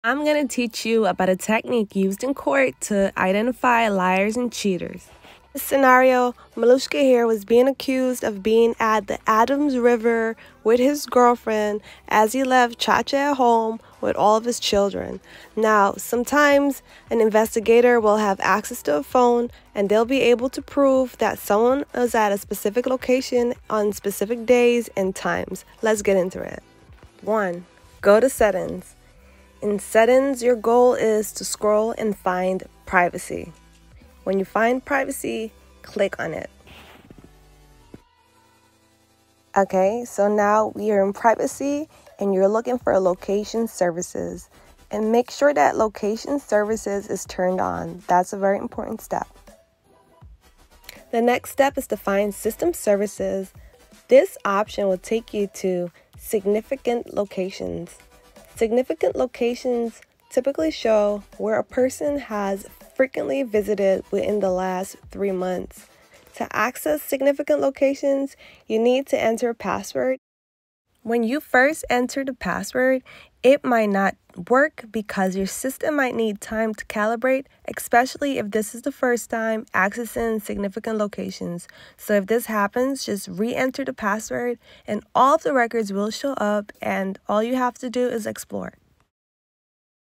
Catch Cheaters and Liars using iPhone
A cybersecurity/digital forensics technique commonly used in court to track locations.
It’s a great tool to understand your technology and it’s capabilities.
Should we build something together using Python, Java, Scalia, JavaScript or C?

Thanks to Miloshka and Chacha for your support. They were great actors.